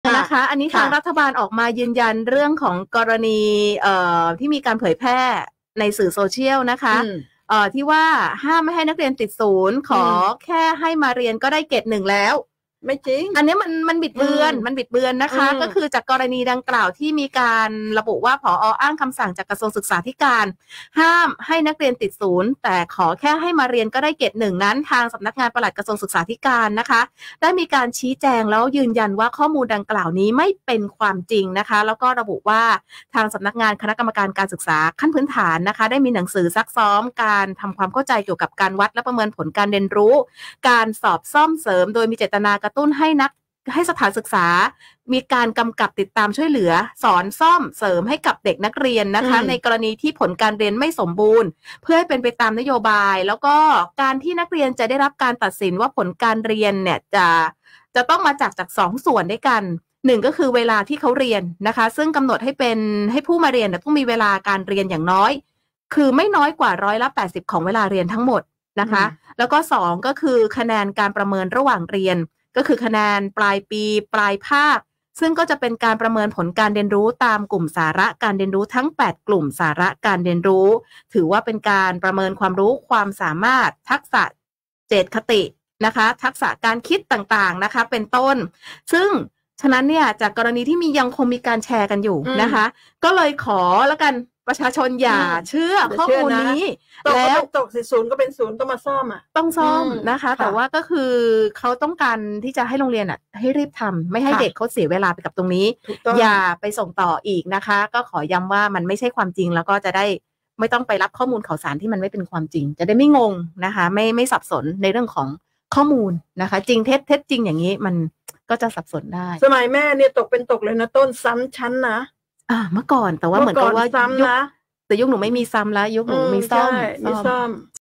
“คารม” ยืนยันข่าว ห้ามให้นักเรียนติด 0 ขอแค่ให้มาเรียนก็ได้เกรด 1 ไม่เป็นความจริง
15 มกราคม 2567 นายคารม พลพรกลาง รองโฆษกประจำสำนักนายกรัฐมนตรี เปิดเผยว่า จากกรณีที่มีการเผยแพร่ว่า ผอ. อ้างคำสั่งจากกระทรวงศึกษาธิการ ห้ามให้นักเรียนติด 0 ขอแค่ให้มาเรียนก็ได้เกรด 1 นั้น  ทางสำนักงานปลัดกระทรวงศึกษาธิการ กระทรวงศึกษาธิการ ได้ชี้แจงแล้วว่า เป็นข้อมูลที่ไม่เป็นความจริง

นายคารม กล่าวว่า จากกรณีดังกล่าว สำนักงานคณะกรรมการการศึกษาขั้นพื้นฐานได้มีหนังสือซักซ้อมความเข้าใจเกี่ยวกับการวัดและประเมินผลการเรียนรู้และการสอนซ่อมเสริมโดยมีเจตนากระตุ้นให้สถานศึกษากำกับ ติดตาม ช่วยเหลือ สอนซ่อมเสริมและดำเนินการวัดและประเมินผลกรณีนักเรียนมีผลการเรียนไม่สมบูรณ์ (ติด 0 ร มส.) เพื่อให้เป็นไปตามนโยบาย เรียนดี มีความสุข ของกระทรวงศึกษาธิการ เพื่อยกระดับคุณภาพการศึกษาทั้งผู้เรียน ผู้สอน  เพื่อให้การเรียนดีขึ้น ลดปัญหาของผู้เรียนในการติด 0 ร มส. ไม่ใช่ห้ามให้นักเรียนติด 0 ตามที่ได้ปรากฎในสื่อ

นายคารม กล่าวว่า การที่นักเรียนจะได้รับการตัดสินผลการเรียน ต้องมาจาก 2 ส่วน ได้แก่ 1) เวลาเรียน ซึ่งกำหนดให้ผู้เรียนต้องมีเวลาเรียนไม่น้อยกว่าร้อยละ 80 ของเวลาเรียน เป็นการมุ่งหวังให้ผู้สอนมีเวลาในการพัฒนาผู้เรียน และเติมเต็มศักยภาพของผู้เรียน และเพื่อให้การจัดการเรียนการสอนมีประสิทธิภาพ และ 2) คะแนนการประเมินระหว่างเรียนและคะแนนปลายปี/ปลายภาค ซึ่งการประเมินผลการเรียนรู้ตามกลุ่มสาระการเรียนรู้ทั้ง 8 กลุ่มสาระการเรียนรู้นั้น เป็นการประเมินความรู้ความสามารถ ทักษะ เจตคติ ทักษะการคิด ที่กำหนดอยู่ในมาตรฐานการเรียนรู้และตัวชี้วัดตามหลักสูตรแกนกลางการศึกษาขั้นพื้นฐาน ซึ่งจะนำไปสู่การสรุปผลการเรียนรู้ของผู้เรียนตามมาตรฐานการเรียนรู้และตัวชี้วัดต่อไป ซึ่งการกำหนดสัดส่วนคะแนนระหว่างเรียนกับคะแนนปลายปี/ปลายภาค นั้นให้ความสำคัญของคะแนนระหว่างเรียนมากกว่าคะแนนปลายปี/ปลายภาค เช่น 60 : 40, 70: 30, 80 : 20 เป็นต้น

“ขอให้ประชาชนอย่าหลงเชื่อข้อมูลดังกล่าว และขอความร่วมมือไม่ส่ง หรือแชร์ข้อมูลดังกล่าวต่อในช่องทางสื่อสังคมออนไลน์ต่าง ๆ และเพื่อให้ประชาชนได้รับข้อมูลข่าวสารจากสำนักงานปลัดกระทรวงศึกษาธิการ กระทรวงศึกษาธิการ สามารถติดตามได้ที่เว็บไซต์ หรือโทร. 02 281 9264” นายคารม ย้ำ